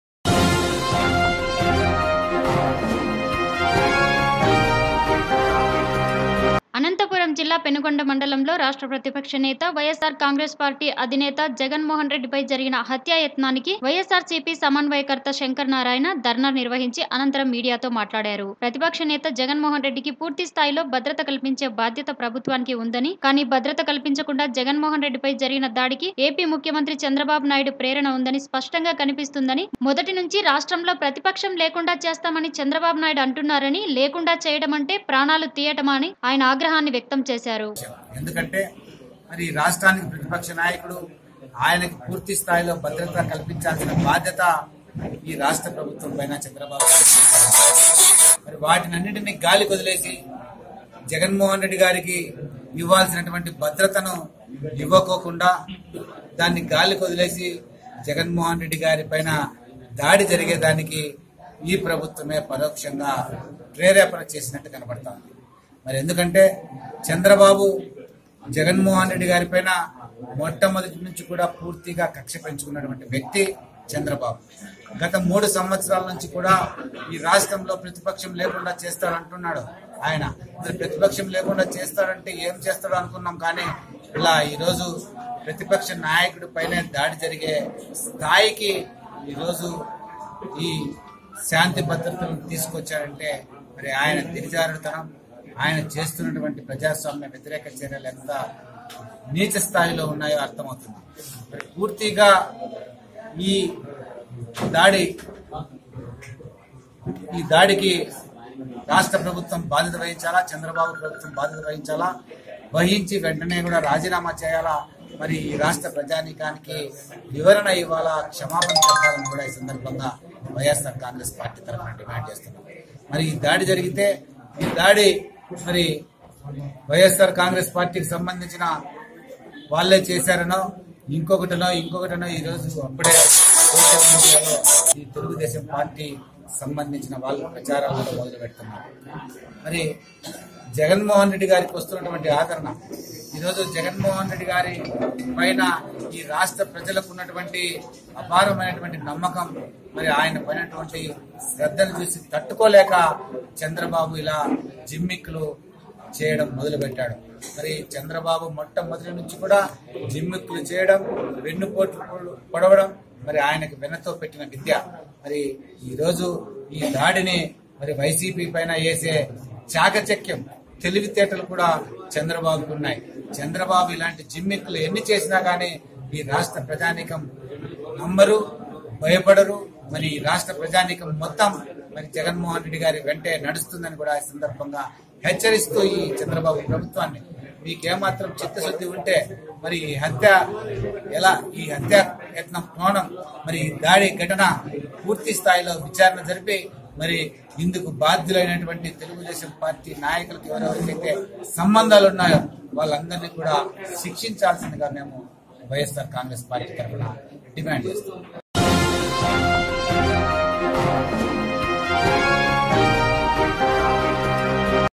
అనంతపురం జిల్లా జగన్ మోహన్ రెడ్డి పై జరిగిన హత్యాయత్నానికి వైఎస్సార్సీపీ //NEWS9TV
అనంతపురం జిల్లా పెనుకొండ మండలంలో. రాష్ట్ర ప్రతిపక్ష నేత వైఎస్సార్ కాంగ్రెస్ పార్టీ అధినేత జగన్ మోహన్ రెడ్డి పై జరిగిన హత్యాయత్నానికి వైఎస్సార్సీపీ సమన్వయకర్త శంకర్ణారాయణ ధర్నా నిర్వహించి అనంతరం మీడియా తో మాట్లాడారు..ప్రతిపక్ష నేత జగన్మోహన్ రెడ్డి కి పూర్తి స్థాయిలో భద్రత కల్పించే బాధ్యత ప్రభుత్వానికి ఉంది..కానీ భద్రత కల్పించకుండా జగన్మోహన్ రెడ్డి పై జరిగిన దాడికి ఏపి ముఖ్యమంత్రి చంద్రబాబు నాయడు ప్రేరణ ఉందని స్పష్టంగా కనిపిస్తుందని మొదటినుంచి రాష్ట్రంలో ప్రతిపక్షం లేకుండా చేస్తాం అని చంద్రబాబు నాయుడు అంటున్నారు లేకుండ చేయటం అంటే ప్రాణాలు తియ్యటమా అని ఆయన వ్యక్తం చేశారు.